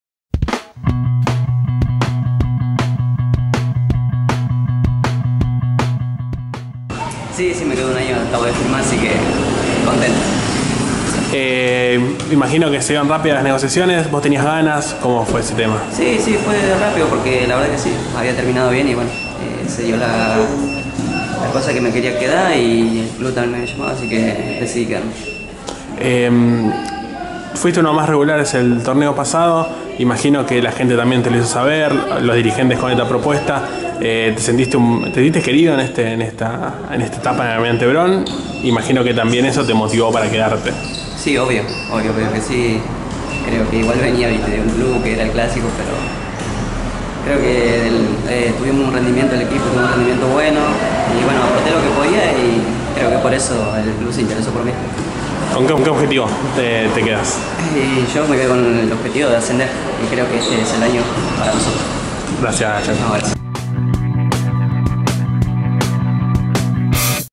Leandro Guzmán, seguirá en Almirante Brown
El volante Leandro Guzmán renovó su contrato con Almirante Brown y seguirá por una temporada más.